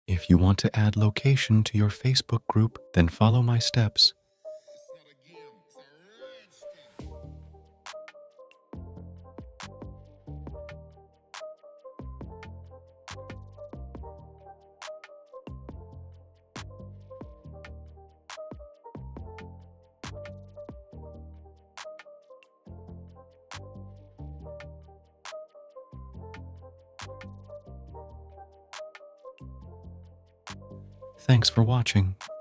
How To Add Location To Facebook Group - How To Easily
Details
if you add location to your facebook group then it is very easy to target your local audience